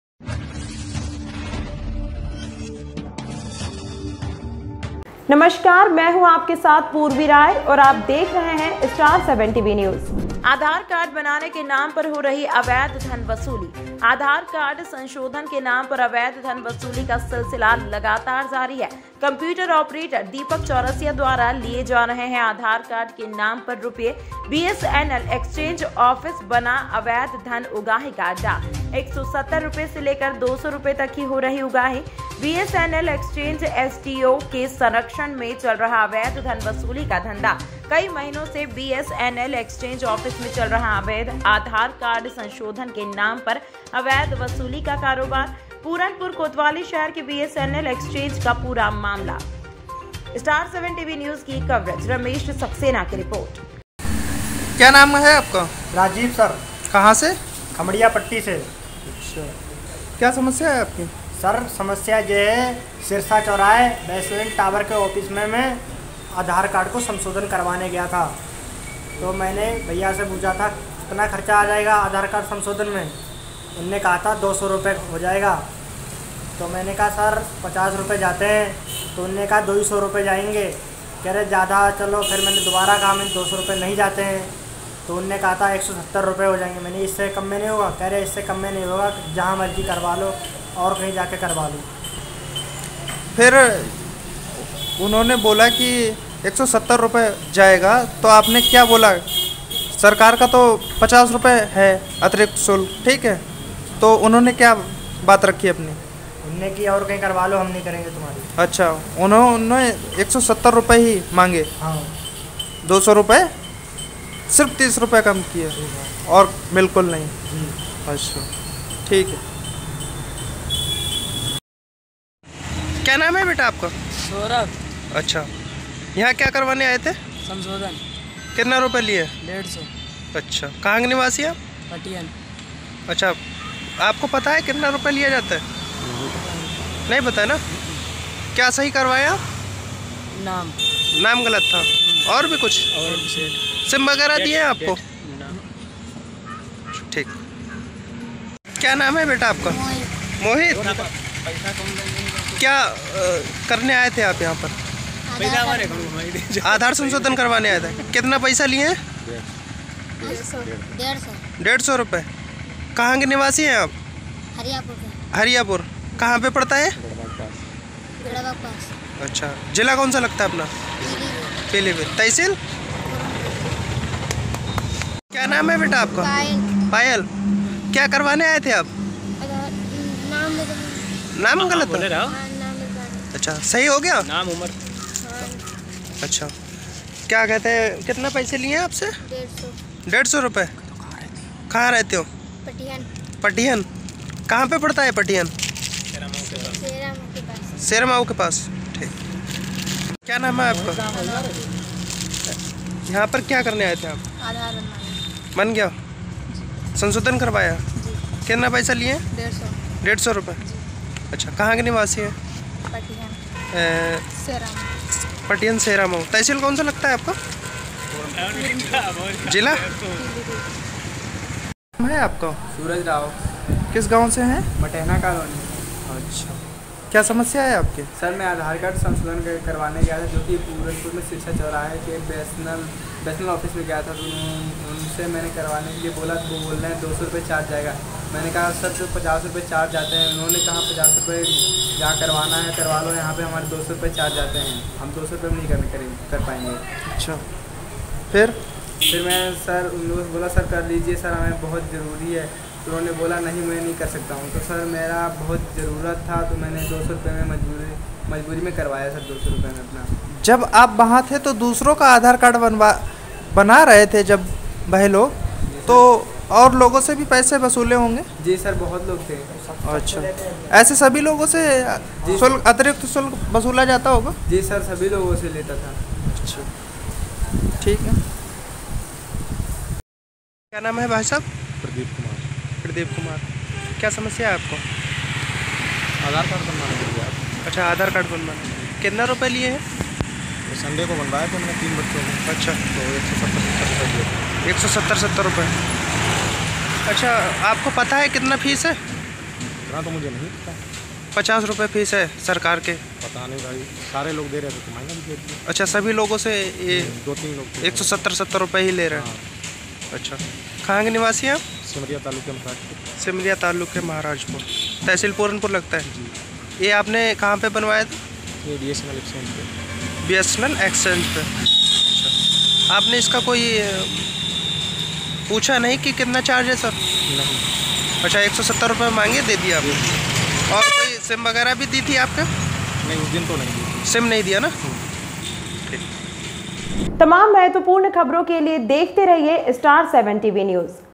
आधार कार्ड बनाने के नाम पर हो रही अवैध धन वसूली
ब्रेकिंग पीलीभीत

आधार कार्ड संशोधन के नाम पर अवैध धन वसूली का सिलसिला लगातार जारी

कम्प्यूटर आपरेटर दिपक चौरसिया द्वारा लिए जा रहे हैं आधार कार्ड के नाम पर  रुपए
बीएसएनएल एक्सचेंज आफिस बना अवैध धन उगाही का अड्डा
170रुपए से लेकर 200 रूपए तक की हो रही उगाही

बीएसएनएल एक्सचेंज एसडीओ के संरक्षण में चल रहा अवैध धन वसूली का धंधा
कई महीनों से बीएसएनएल एक्सचेंज आफिस में चल रहा आधार कार्ड ससोधन के नाम पर अवैध वसूली का कारोबार
पूरनपुर कोतवाली शहर के बीएसएनएल एक्सचेंज का पूरा मामला
रमेश सक्सेना की रिपोर्ट